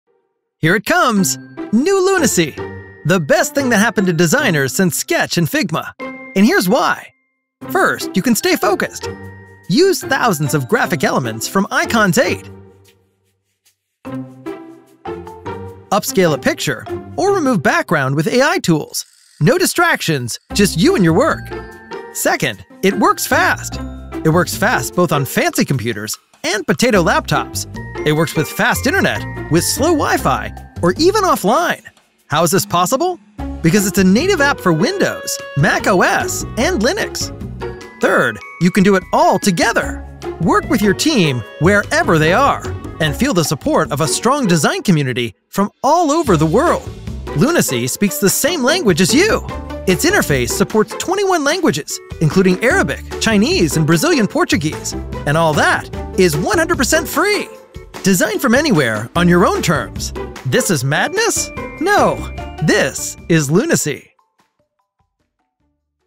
Lunacy 8 - Free design tool. Faster than Figma. Smarter than Sketch.
Lunacy is a free collaborative design tool for web and UX/UI design. It has everything you are used to in other similar apps and goes beyond. Lunacy will speed up design workflow and minimize distractions. Runs on Windows, macOS, and Linux. Native apps for all platforms.

Key highlights:
- Built-graphics at your fingertips: icons, illustrations, and photos
- AI-based tools onboard: background remover, image upscaler, avatar and text generators
- UI kits: Microsoft, Google, and Apple
- Works both with cloud and local documents
- Works without internet
- Translated in 20+ languages
- Dark theme